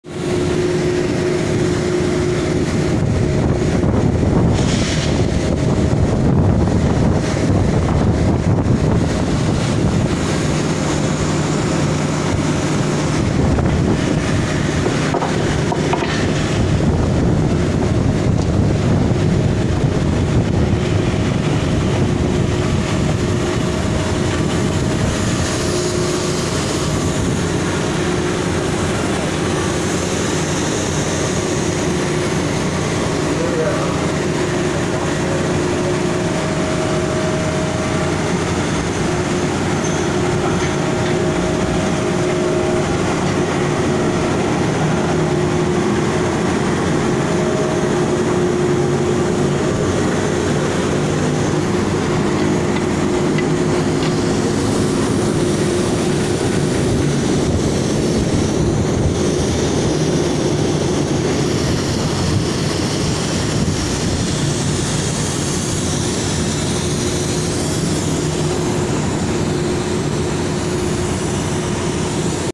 Derrickman life working on derrick drill pipe Running, open hole slow running opration.oil and gas.
Derrick man working on derrick, drill pipe (Running in hole), open hole slow running  opration.